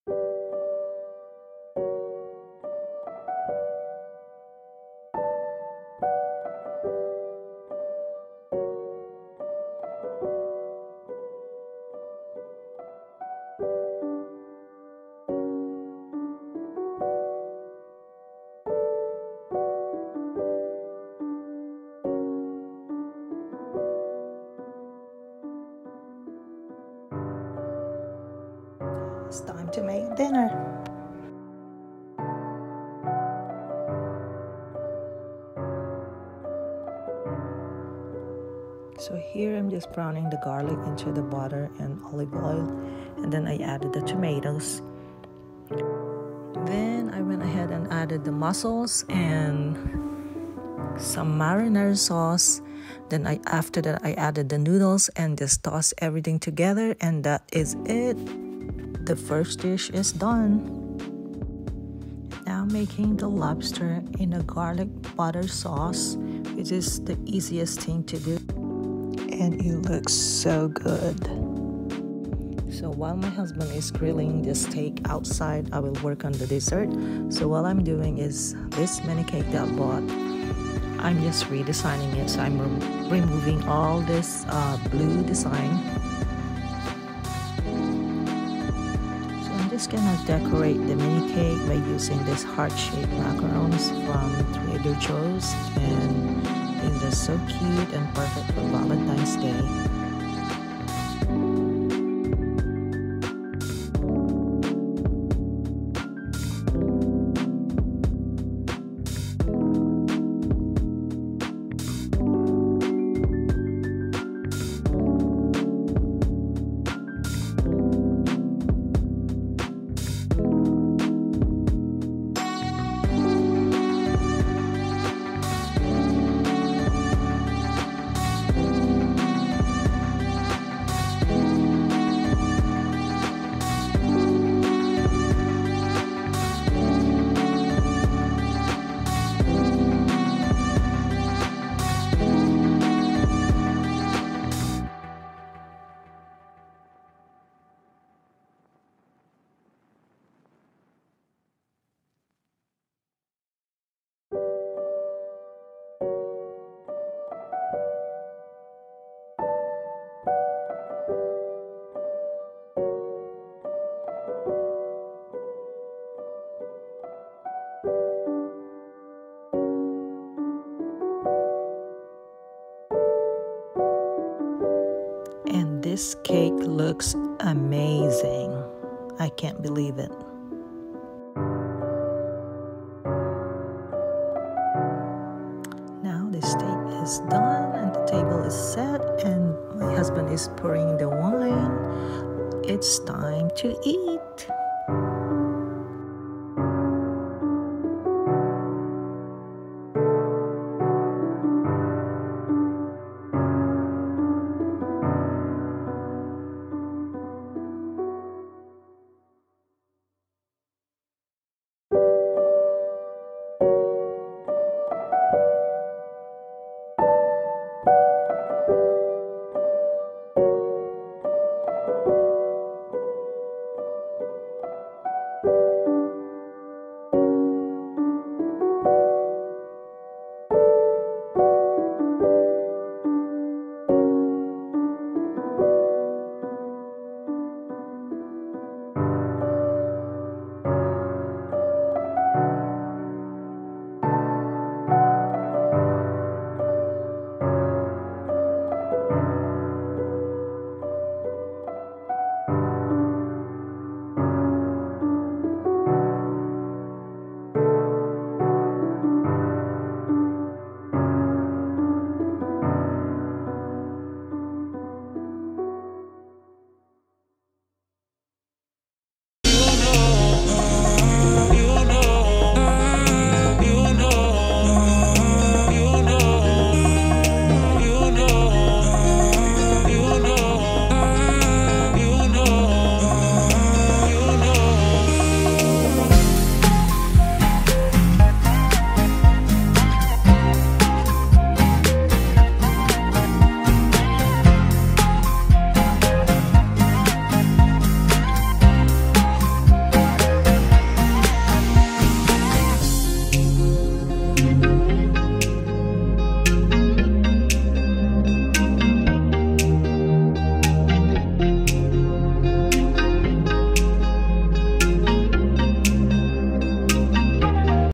Our Valentine’s Day dinner: Steak, lobster, pasta with mussels in a white wine sauce and mini cake
We are sharing how we made our Valentine’s Day dinner date at home very special. Surprisingly, all the food was all easy to make but they tasted so good!

This is how we usually spend our Valentine’s Day as we hate to go in a crowded restaurant on a special day  such as this. With a little bit of planning and prepping anyone can do this.